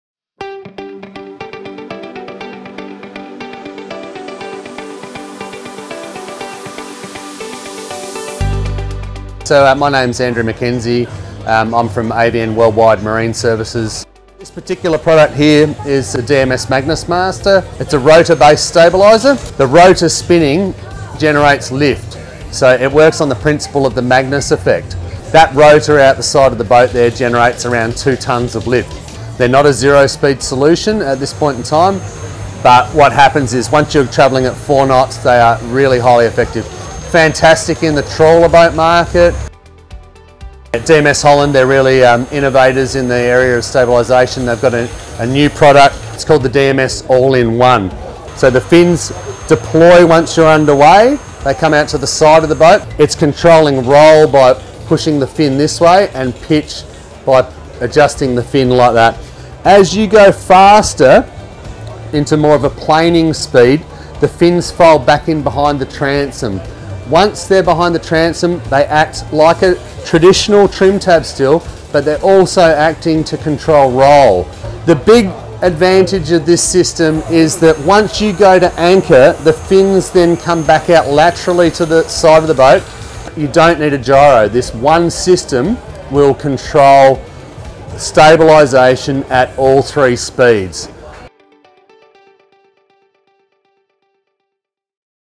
DMS Holland Stabilisers | Club Marine TV First Look
You've seen the Magnus Effect when someone drops a spinning basketball from a height and DMS Holland is now using it for rotary stabilisation of trawler designs. Watch our First Look from Melbourne Boat Show 2024 for more details.